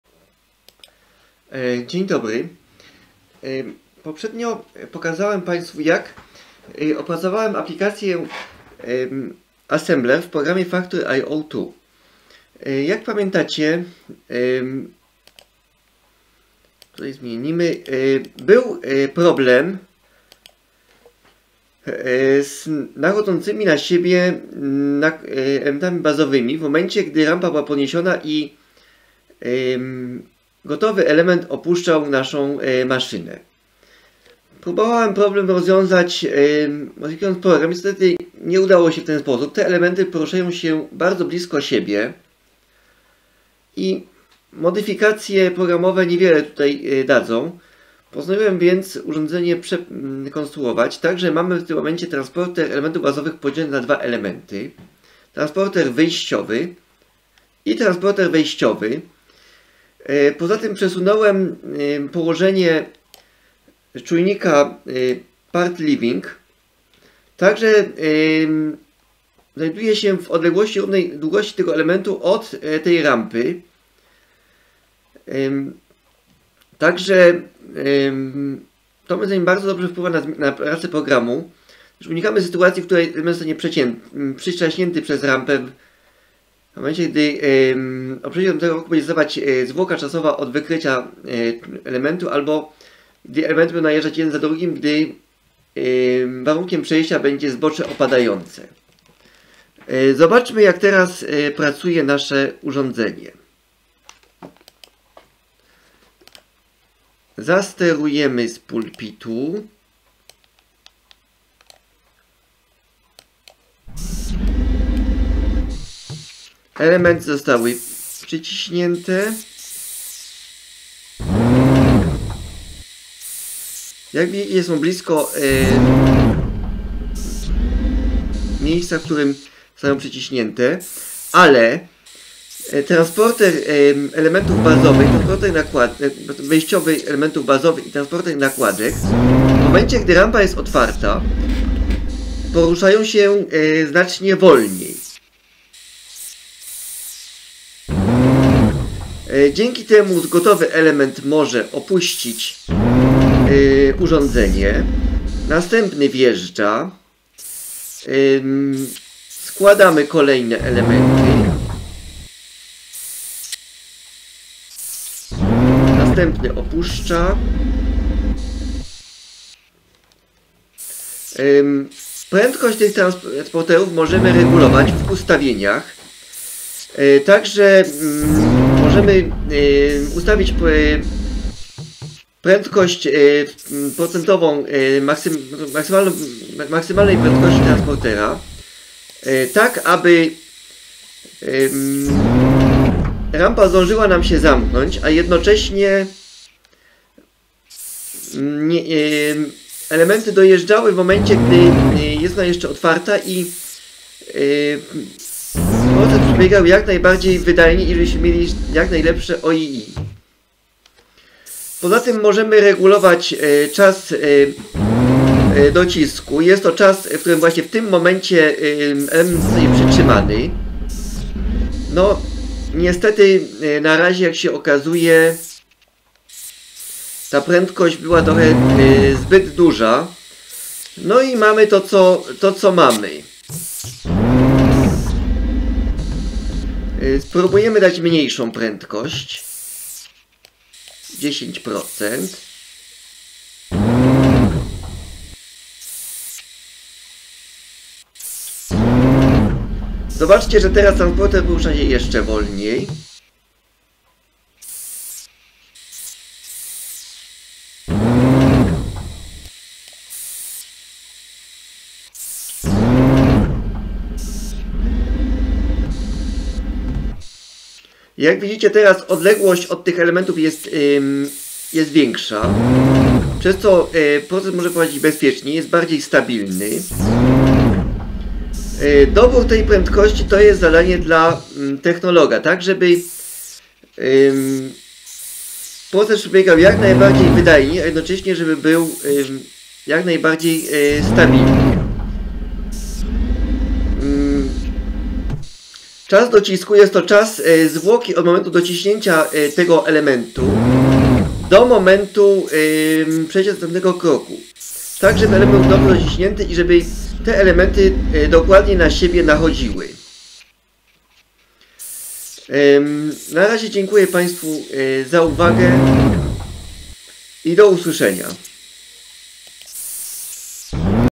Assembler Factory I/O2 zmodyfikowany
Dzień dobry
Poprzednio zaprezentowałem Państwu jak działa opracowany przeze mnie program sterujący aplikacją Assembler. Niestety problemu dopuszczających urządzenie niezłożonych elementów bazowych nie udało się rozwiązać zmianami w programie. Problem rozwiązałem modyfikując urządzenie. Podzieliłem transporter elementów bazowych na dwa odcinki, wejściowy i wyjściowy. Prędkość transportera wejściowego i nakładek są regulowane. Gdy rampa jest otwarta poruszają się wolniej. Prędkość można regulować tak by zoptymalizować proces pod kątem wydajności. Ponadto przesunąłem czujnik Part leaving, tak by znajdował się w odległości od rampy równej długości detalu.
Pozdrawiam i zapraszam do współpracy na polu szeroko pojętej automatyki.
Piotr Kania